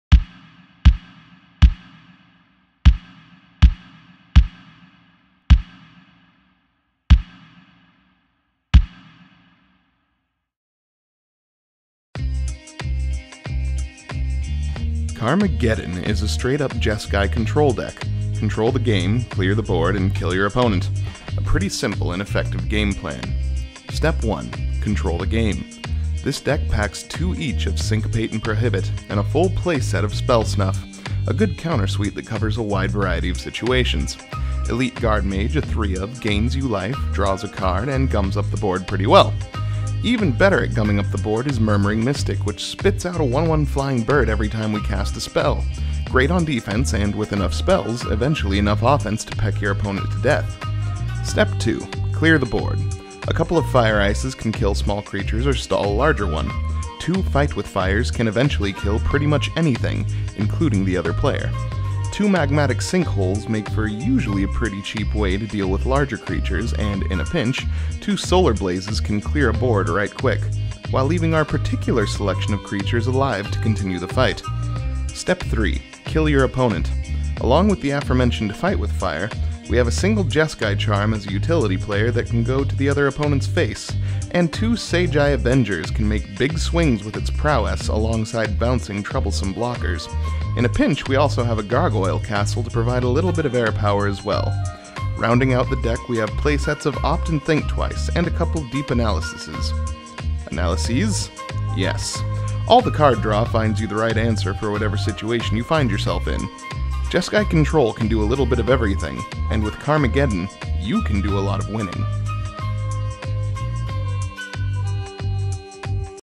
[MTG[ Battle Deck Deck Tech - Karmageddon
Jeskai Control has a pretty simple game plan: Control the board, clear the way, and kill your opponent. With Karmageddon, you'll get all the tools you need to control your destiny...and control your opponent out of the game!

*NOTE: Battle Deck Decklists shift a bit over time as card availability changes - the deck will always function off the same core idea, but the specifics might be a bit different from the initial run of the deck.

Get your hands on this and all our other Battle Decks